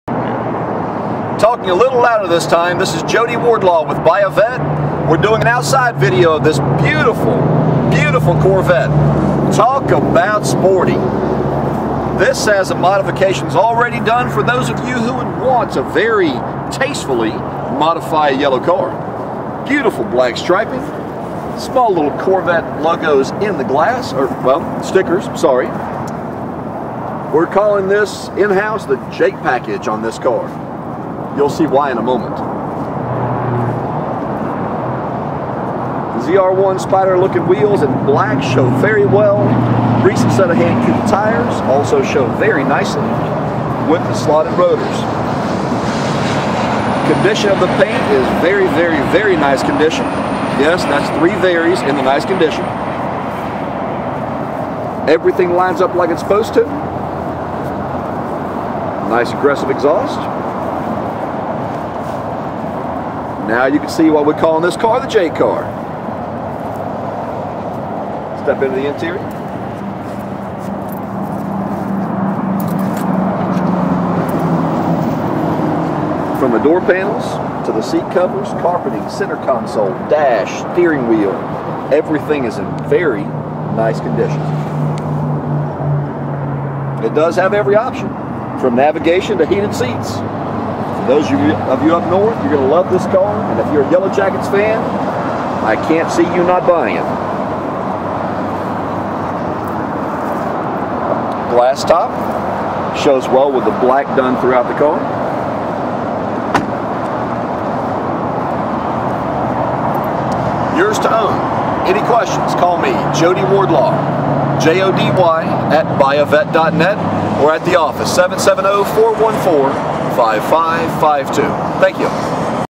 2007 Corvette Z51
2007 Corvette Z51 For Sale

Velocity Yellow/Black exterior, Ebony/Yellow interior, glass top. 410hp LS2 engine, 6 speed manual transmission, posi rear-end. LOADED with nearly every available factory option, this 2007 Corvette Z51 is in nice condition with 38,084 miles. This car's paint is in very good condition, with a deep shine. Black stripes have been painted onto this car (one in the rear, three in the front), and the b-pillar has been painted black to resemble the 1988 35th Anniversary Corvette. Rear bumper stripe has been cutout to create a Jake logo. Glass top is in very good to excellent condition with no cracks or de-lamination. Added black C6 ZR1 style wheels are in very good condition, and wear a set radial tires, showing an average of 6/32nds tread depth remaining in the front, and an average of 5/32nds tread depth remaining in the rear. The interior of this 2007 Corvette Z51 shows very good condition factory leather seat covers, door panels, and center console, and a good condition steering wheel. Other additions to this car include a wraparound flush-fit painted rear spoiler, LS2 hood badges, black Z51 suspension badges for the fenders and rear bumper, "Corvette" vinyl script for the headlights, polished rear bumper fill-in, and a performance catback exhaust system. Subframe is corrosion-free. Please note that this car does have a Carfax blemish: it was involved in a rear impact in Georgia in 2010, with minor damage reported. This car has been fully serviced, and all instrument gauges, lights, turn signals, brakes, engine, transmission and rear-end are all in good operating condition. Call us before this one's sold! Factory features include the $4,945 3LT Preferred Equipment Group (Heads Up Display, memory package, tilt-n-tele, cargo net, auto-dimming mirrors, compass, dual power heated sport seats with airbags, universal garage door transmitter), Bose CD/XM/MP3 stereo with steering wheel controls and Navigation, OnStar, 6 speed manual transmission, Velocity Yellow tintcoat upcharge, Z51 Performance Handling package, glass top, HID headlights, dual zone electronic climate control, fog lamps, keyless entry and start, electric rear hatch pull-down, power windows, power locks, power steering, power brakes, power mirrors, cruise control, ABS, traction control, Active Handling. 38K Miles.